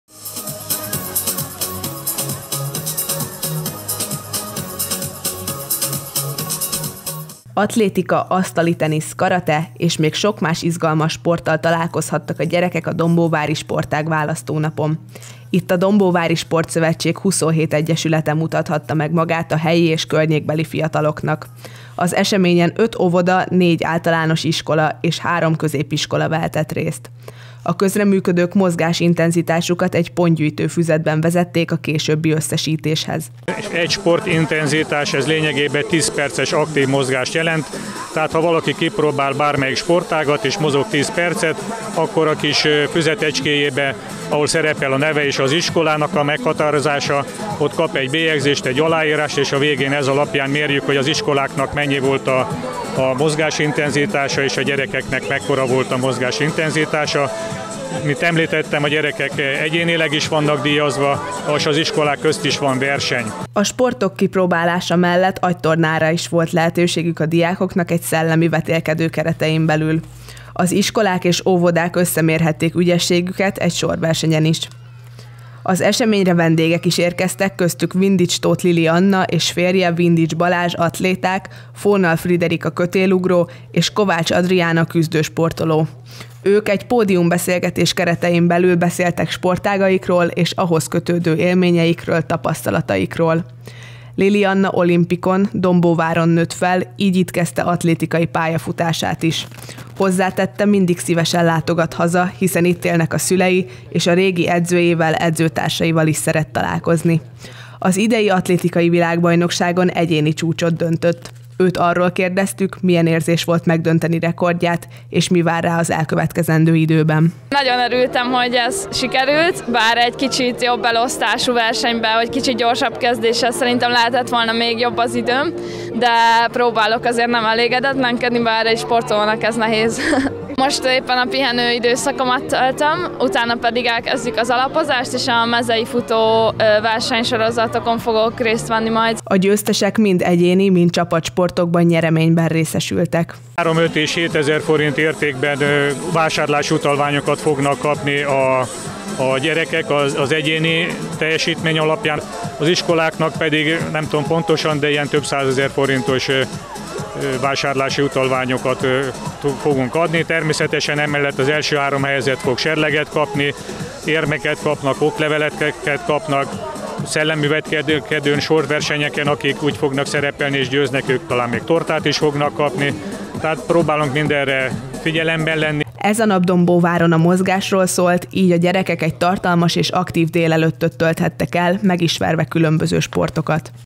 Sportág választó Dombóváron - különleges sportágak gyerekeknek
Ismét megrendezték Dombóváron a sportágválasztó gyerek napot. Ezt a hagyományőrző eseményt évek óta az iskolakezdés után rendezik meg. A nap során a gyerekek megismerkedhettek a város sportegyesületeivel, és lehetőségük volt kipróbálni magukat a legkülönlegesebb sportágakban is.